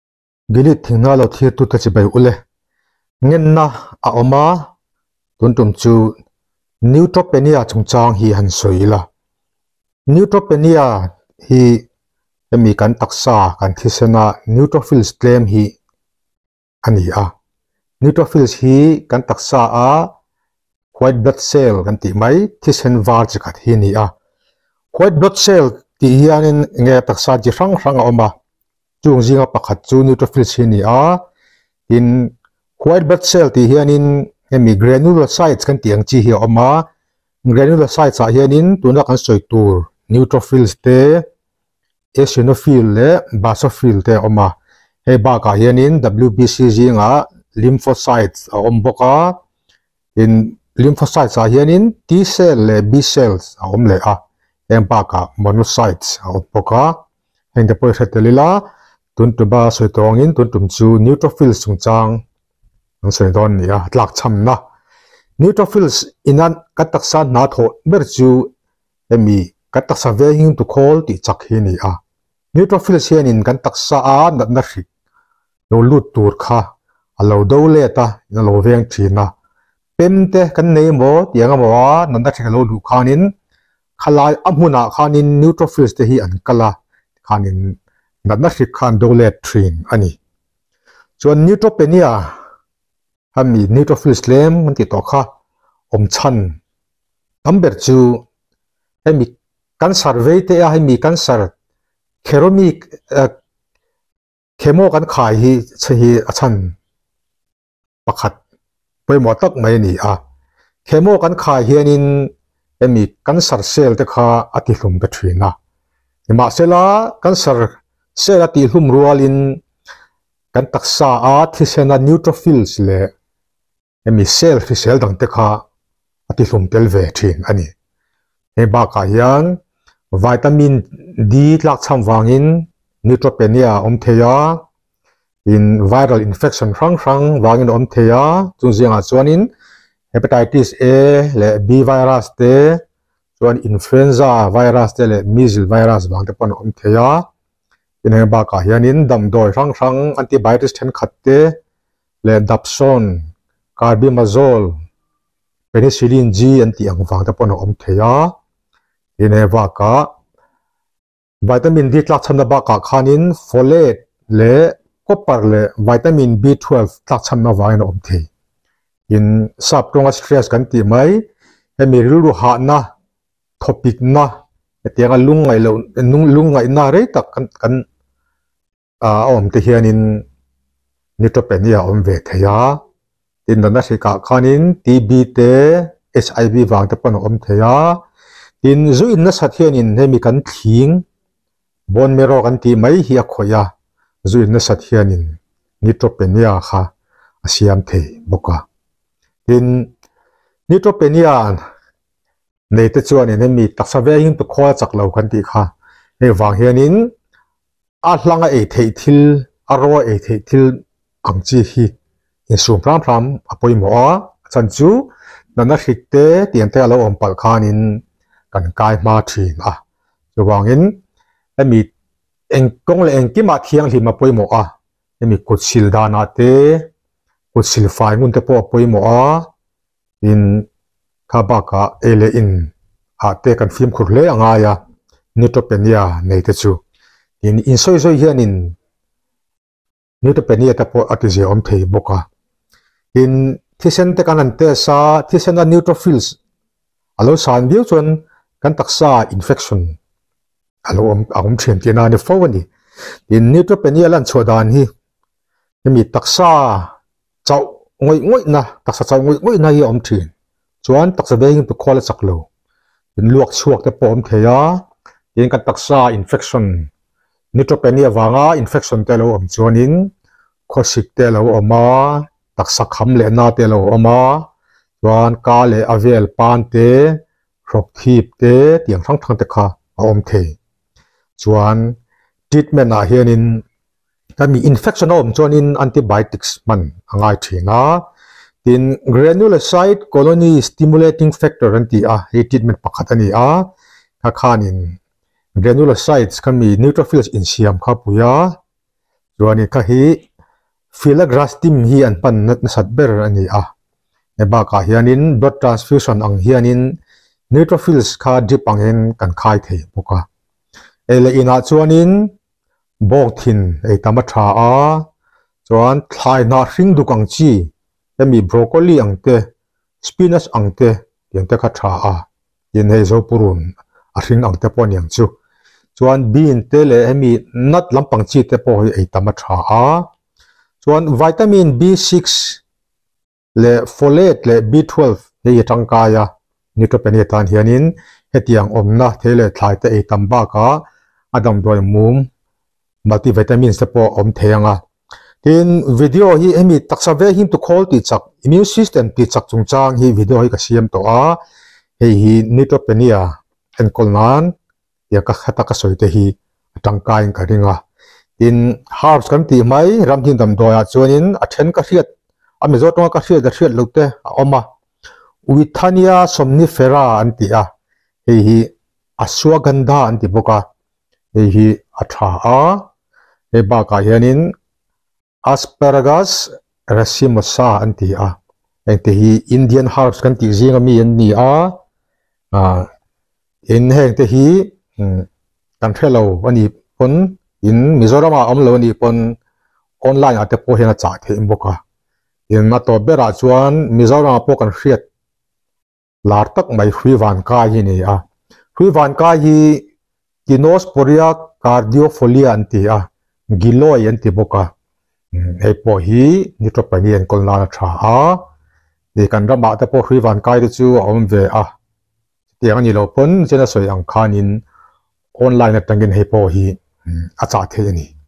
Neutropenia nei rin aiin kan tam
Neutropenia hi taksaa thisen var chi khat tlem hi a ni a, rin aiin nei kan tam. A hrilhfiahna, a awm chhan, a lan chhuah dan, invenna leh inenkawlna te han tar lang teh ang. (@Gilead-Thinghnai)